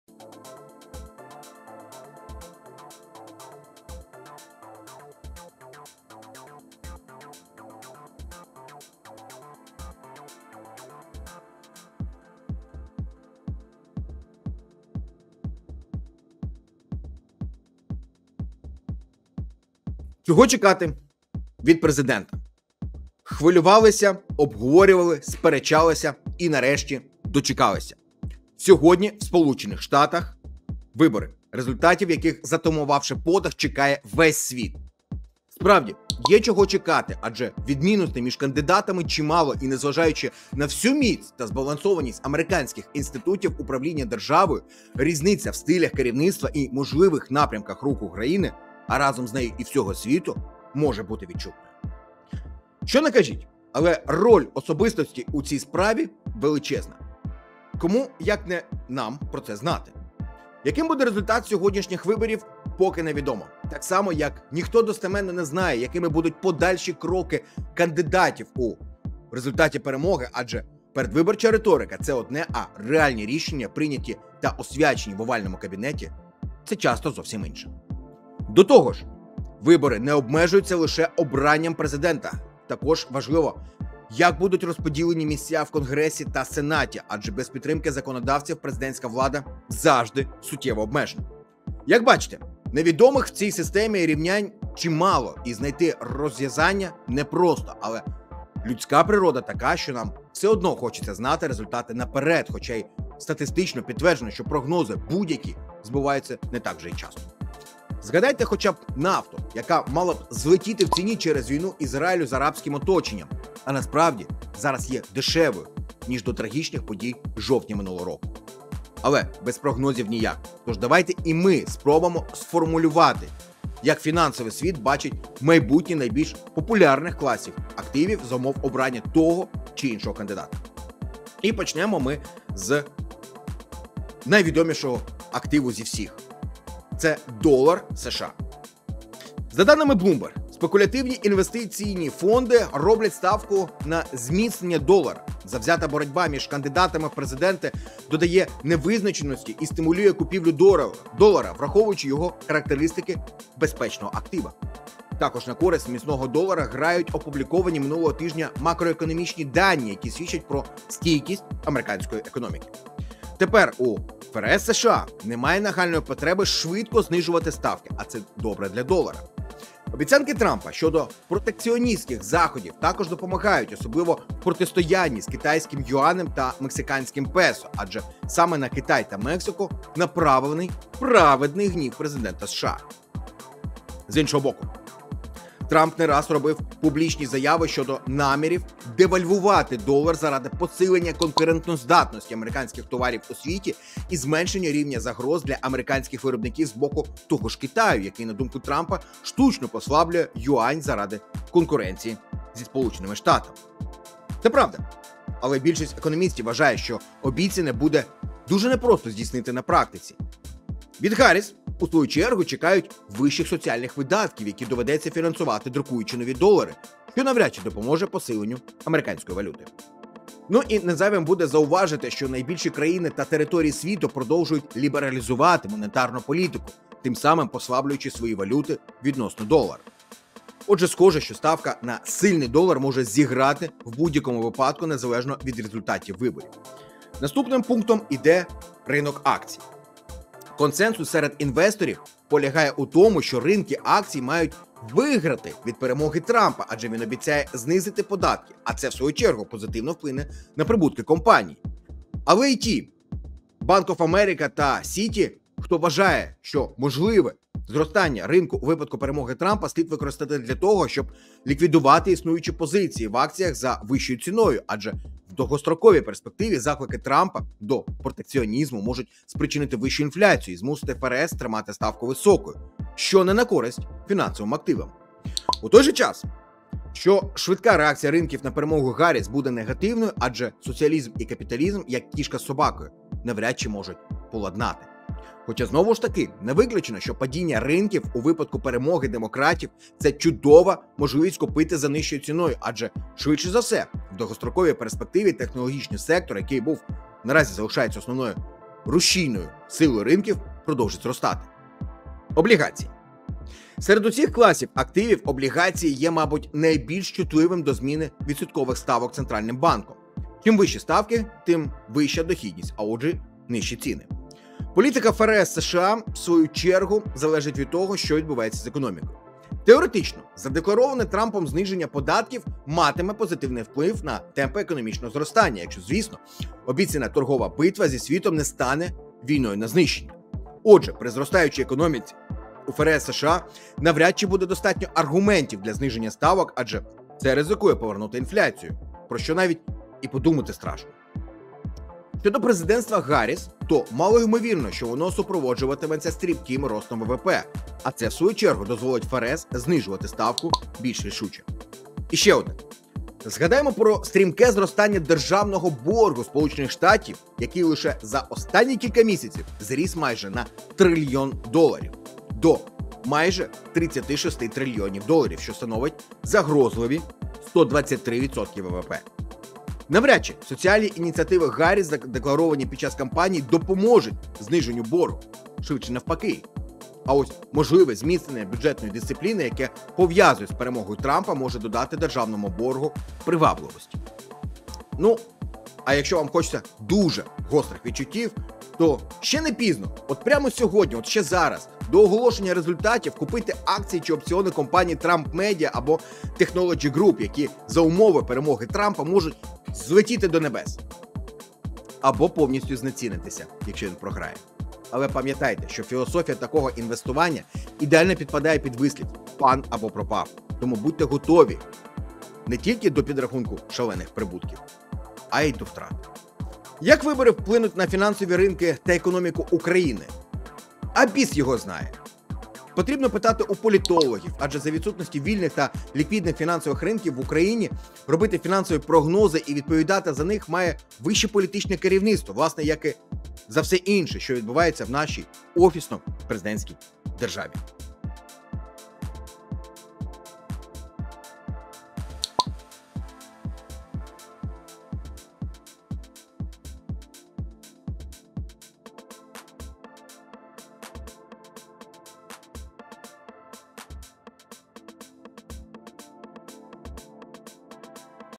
ЩОЙНО ! Трамп ПОДЗВОНИВ ПУТІНУ! Чого ТЕПЕР чекати?
Чого чекати від президента?
Хвилювалися, обговорювали, сперечалися і нарешті дочекалися.
Сьогодні в США вибори, результатів яких затамувавши подих чекає весь світ
Справді є чого чекати, адже відмінностей між кандидатами чимало і незважаючи на всю міць та збалансованість американських інститутів управління державою, різниця в стилях керівництва і можливих напрямках руху країни, а разом з нею і всього світу, може бути відчутною.
Що не кажіть, але роль особистості в цій справі величезна.
Кому як не нам про це знати?